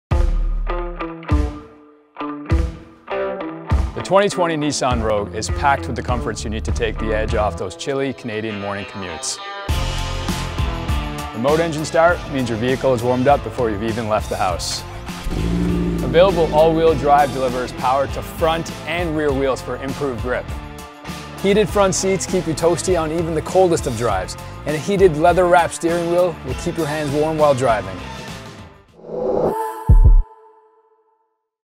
2020 Nissan Rogue Walk Around | Cold Weather Ready
The 2020 Nissan Rogue is available with features such as Remote Engine start, Intelligent All-Wheel Drive, Heated Front Seats and Heated leather-wrapped steering wheel, that allow you to enjoy a more confident and comfortable ride. With tasteful touches throughout, this is one natural, relaxed and decidedly upscale environment.

To learn more about the 2020 Nissan Rogue, visit us